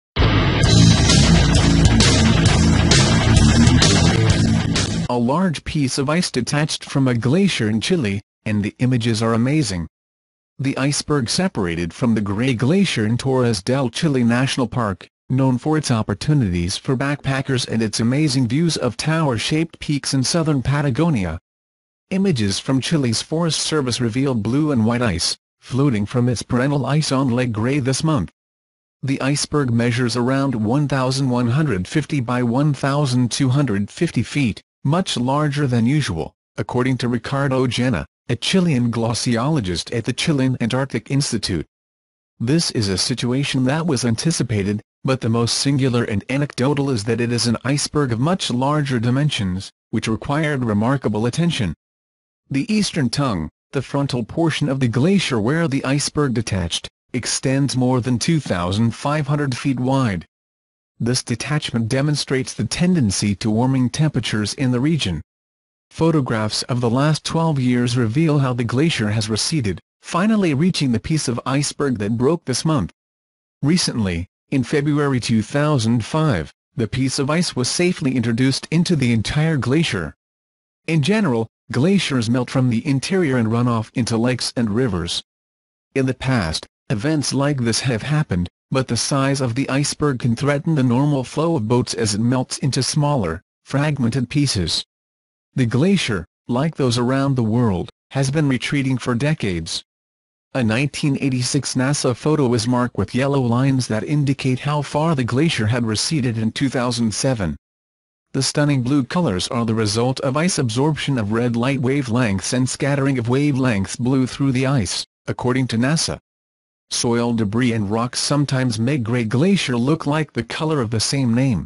GIANT ICEBERG HAS BREAKED THE GRAY GLACIER OF CHILE.
A large piece of ice detached from a glacier in Chile, and the images are amazing. The iceberg separated from the Gray Glacier in Torres del Chile National Park, known for its opportunities for backpackers and its amazing views of tower-shaped peaks in southern Patagonia. Images from Chile's forest service reveal blue and white ice, floating from its parental ice on Lake Gray this month. The iceberg measures around 1,150 by 1,250 feet, much larger than usual, according to Ricardo Jaña, a Chilean glaciologist at the Chilean Antarctic Institute. This is a situation that was anticipated, but the most singular and anecdotal is that it is an iceberg of much larger dimensions, which required remarkable attention. The eastern tongue, the frontal portion of the glacier where the iceberg detached, extends more than 2,500 feet wide. This detachment demonstrates the tendency to warming temperatures in the region.

Photographs of the last 12 years reveal how the glacier has receded, finally reaching the piece of iceberg that broke this month. Recently, in February 2005, the piece of ice was safely introduced into the entire glacier. In general, glaciers melt from the interior and run off into lakes and rivers. In the past, events like this have happened, but the size of the iceberg can threaten the normal flow of boats as it melts into smaller, fragmented pieces. The glacier, like those around the world, has been retreating for decades. A 1986 NASA photo is marked with yellow lines that indicate how far the glacier had receded in 2007. The stunning blue colors are the result of ice absorption of red light wavelengths and scattering of wavelengths blue through the ice, according to NASA. Soil debris and rocks sometimes make Gray Glacier look like the color of the same name.